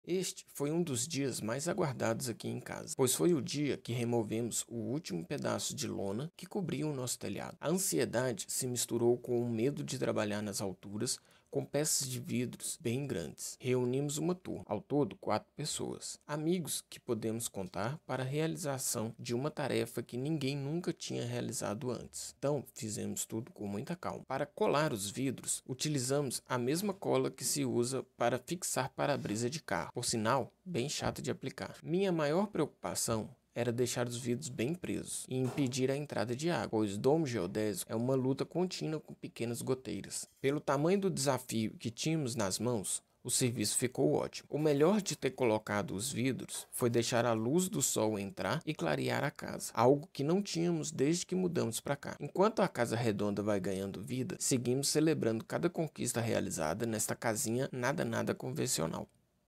Colocando Vidro no Domo Geodésico
Este dia foi uma conquista em tanto, pois além de colocar luz dentro de nossa casa, removemos o ultimo pedaço de lona que cobria nosso telhado instalando os vidros no domo geodésico.